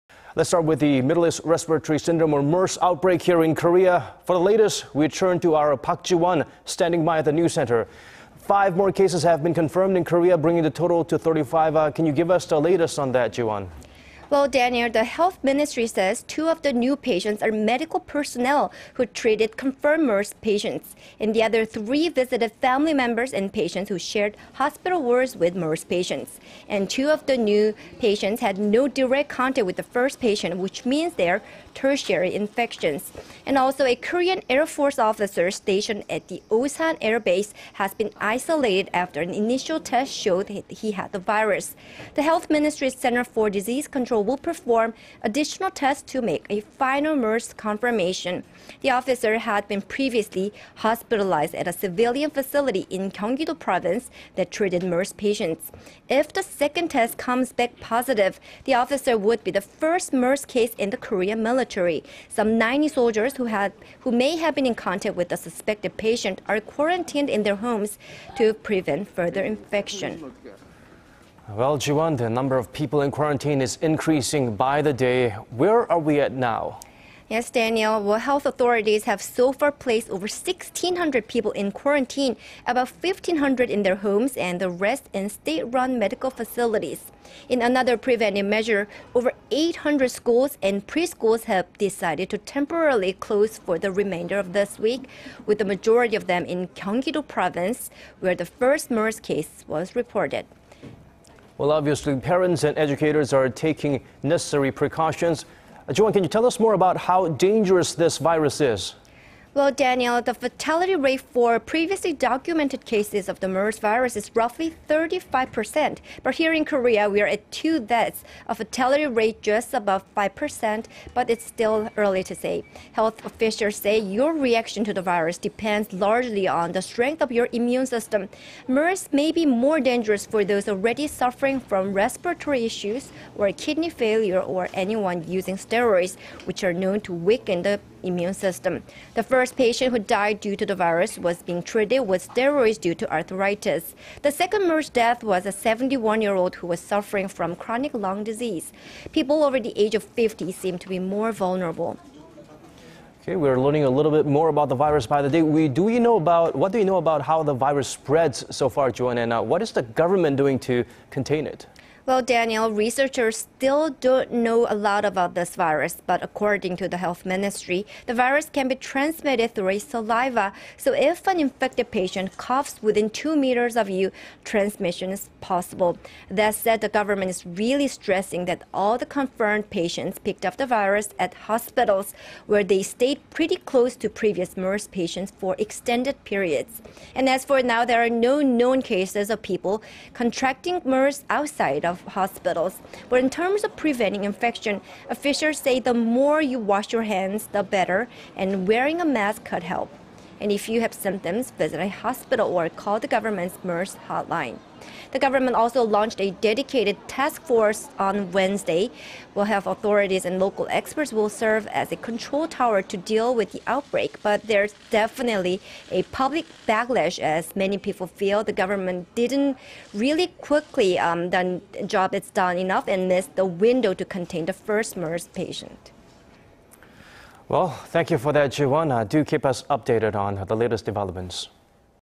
35 confirmed MERS cases as of Thursday afternoon 메르스 6층 연결
We start with the Middle East Respiratory Syndrome or MERS outbreak in Korea.
For the latest， let′s connect with our Park Ji－won from the news center.
So Jiwon， five more cases have been confirmed in Korea... bringing the total to 35.
Well， Daniel...
The health ministry says，.. two of the new patients are medical personnel who treated confirmed MERS patients.
The other three visited family members and patients who shared hospital wards with MERS patients.
And two of the new patients had no direct contact with the first patient， which means they are tertiary infections.
Also， a Korean Air Force officer stationed at the Osan Air Base has been isolated after an initial test showed he had the virus.
The Health Ministry′s Center of Disease Control will perform additional tests to make a final MERS confirmation.
The officer had been previously hospitalized at a civilian facility in Gyeonggi－do Province that treated MERS patients.
If the second test comes back positive， the officer would be the first MERS case in the Korean military.
Some 90 soldiers who may have been in contact with the suspected patient are quarantined in their homes... to prevent further infection.
And Jiwon， the number of people in quarantine is increasing daily.
Where are we at now？
Yes， Daniel...
health authorities have so far placed over 16－hundred people in quarantine，... about 15－hundred in their homes，... and the rest in state－run medical facilities.
In another preventative measure， over eight－hundred schools and preschools have decided to temporarily close for the remainder of this week，... with the majority of them in Gyeonggi－do Province where the first MERS case was reported.
Parents and educators are clearly taking precautions.
Tell us more about how dangerous the virus is.
Well， Daniel，... the fatality rate for previously documented cases of the MERS virus is roughly 35－percent， but here， in Korea， we′re at two deaths，... a fatality rate just above five percent， but it′s still early.
Health officials say，... your reaction to the virus depends largely on the strength of your immune system.
MERS may be more dangerous for those already suffering from respiratory issues or kidney failure or anyone using steroids， which are known to weaken the immune system.
The first patient who died due to the virus was being treated with steroids due to arthritis.
The second MERS death was a 71－year－old who was suffering from chronic lung disease.
People over the age of 50 seem to be more vulnerable.
What do we know about how the virus spreads，... and what is the government doing to contain it？
Well， Daniel... researchers still don′t know a lot about this virus.
But， according to the health ministry，... the virus can be transmitted through saliva，... so if an infected patient coughs within two meters of you，... transmission is possible.
That said，... the government is really stressing that all the confirmed patients picked up the virus at hospitals，